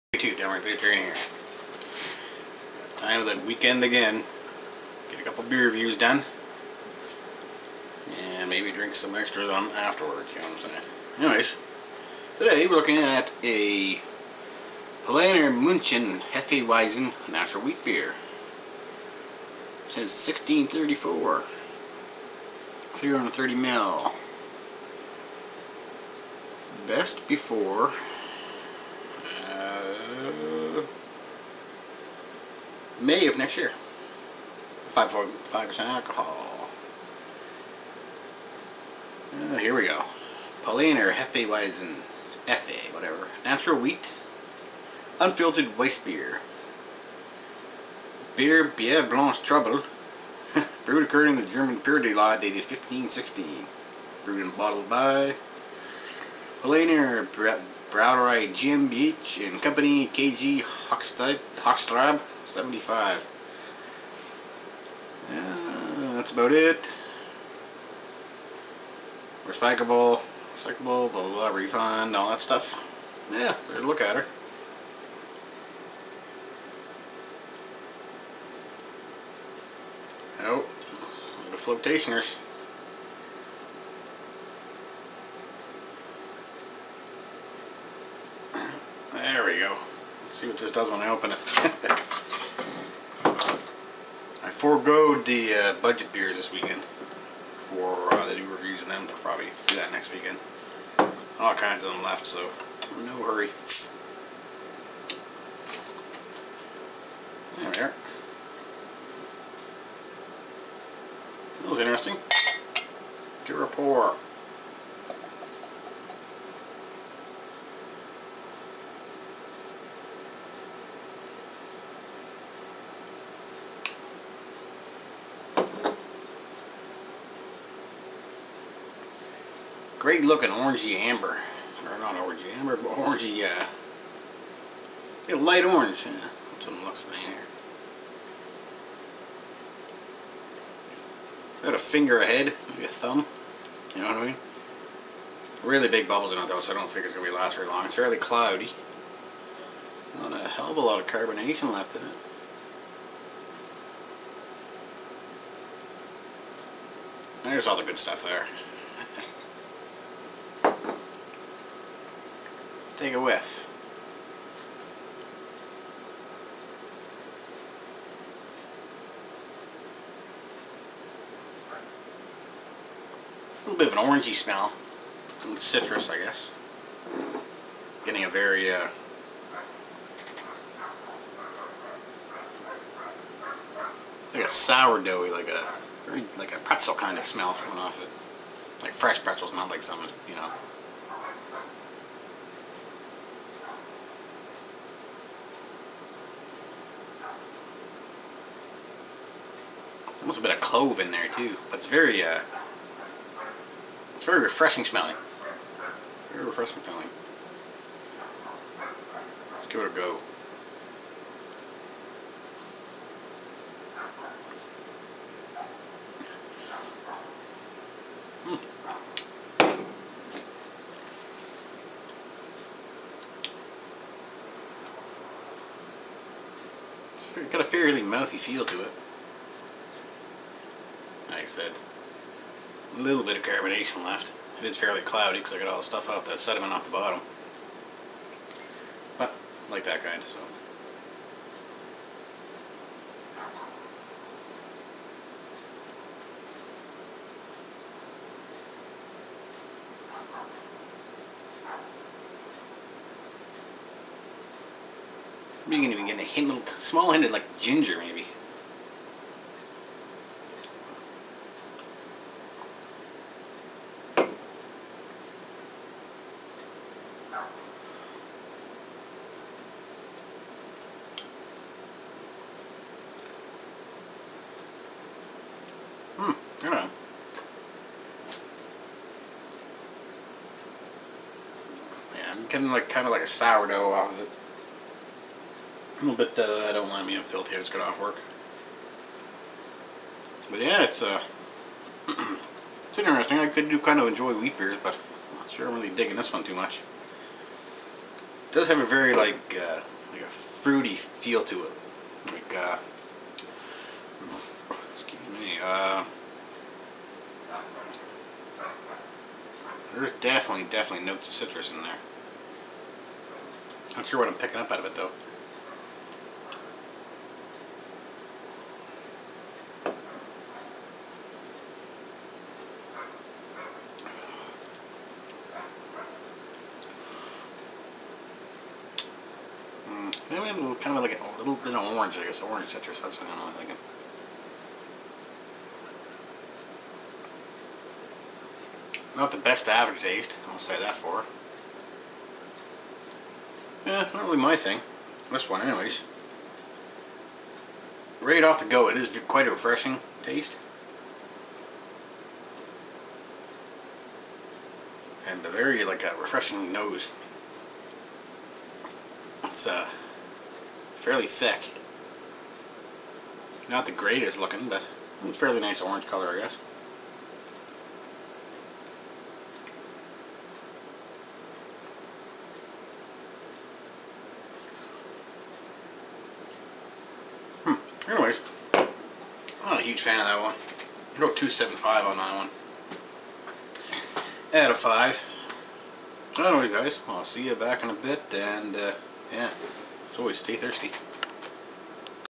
Paulaner Hefe-Weizen Natural Wheat - Beer Review #37
Another wheat brew...usually enjoy these ones...let check out this one!